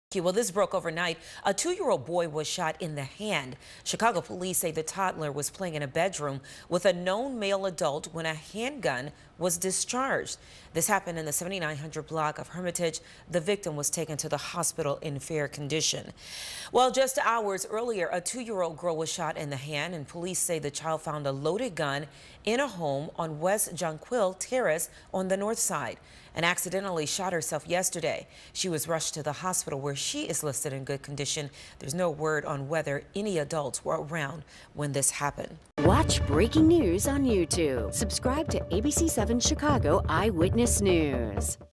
Two 2-year-olds shot hours apart in Chicago
Two toddlers were injured in Chicago shootings Sunday on the North and South sides. FULL STORY